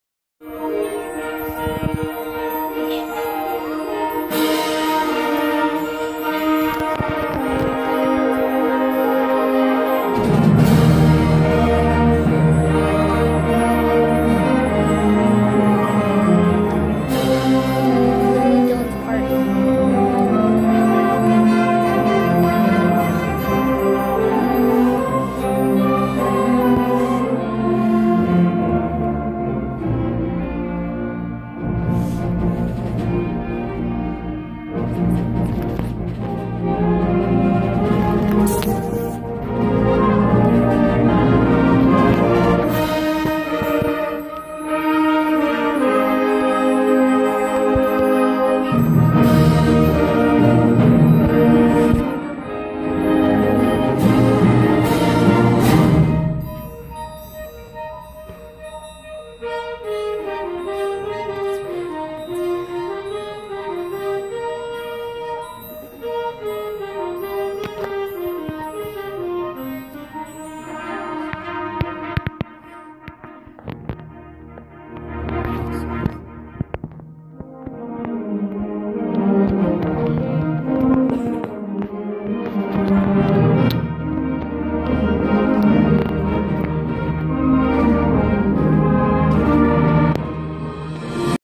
Tony hillerman middle school band-Prefestival (why trombonist should never record)
*cough cough*
I was playing and my brother and his friend (both trombonist sadly) attempting to record us playing.